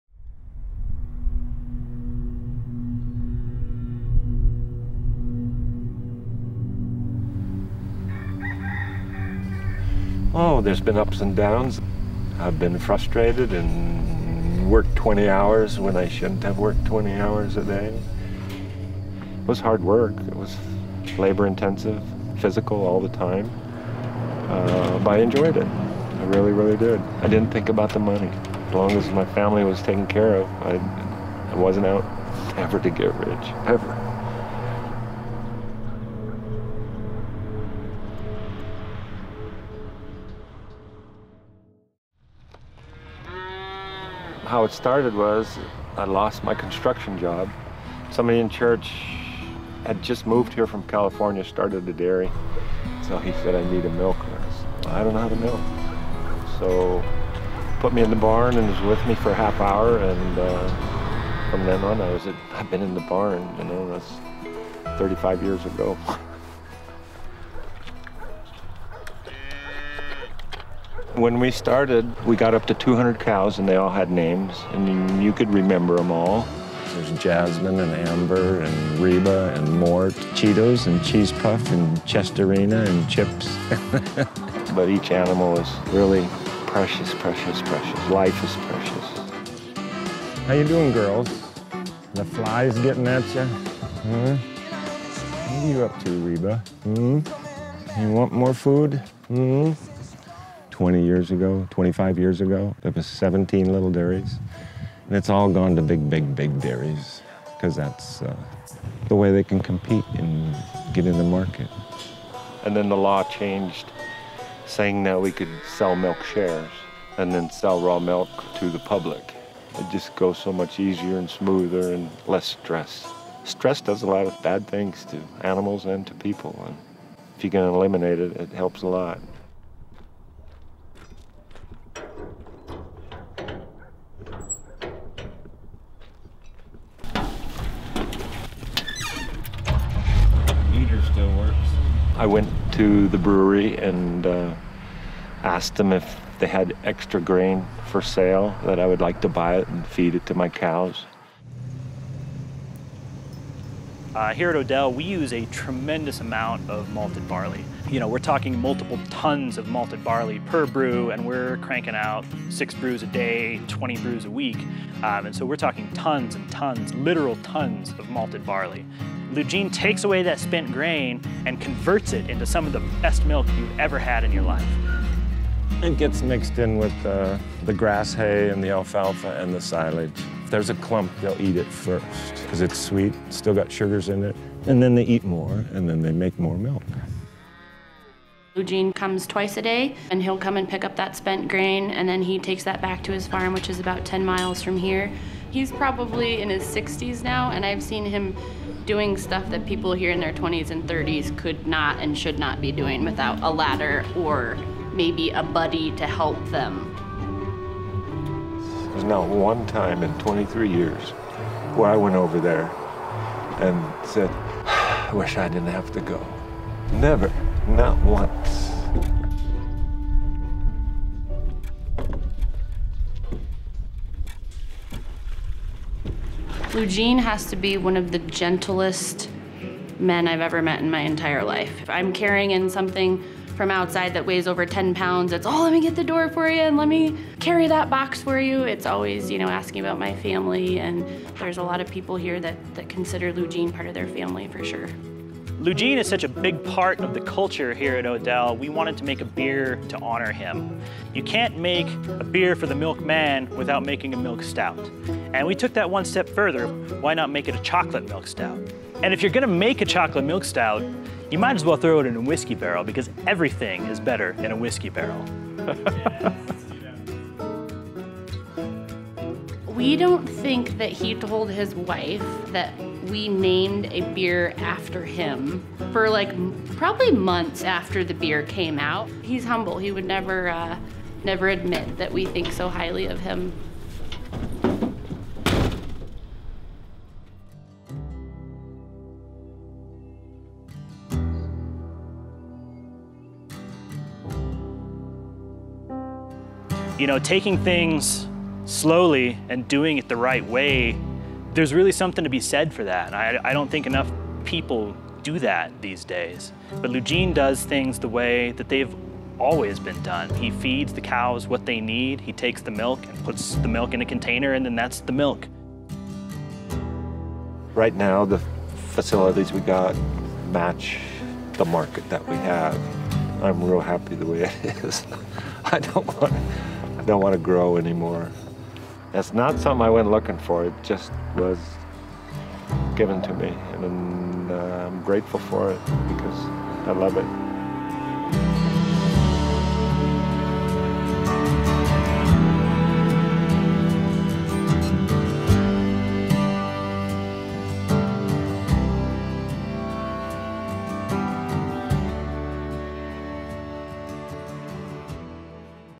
Odell Brewing Presents The Dairyman
If you've spent time here at the brewery, you've likely seen Lugene and his old blue pickup. For the last 23 years, he's been hauling away our spent grain and feeding it to his dairy cows. As an independent dairy farmer, he's been selling his unpasteurized milk directly to the Fort Collins community for decades. Whiskey Barrel-aged Lugene is available in 12oz bottles across out 15-state distribution.

Produced by: Reel Motion Media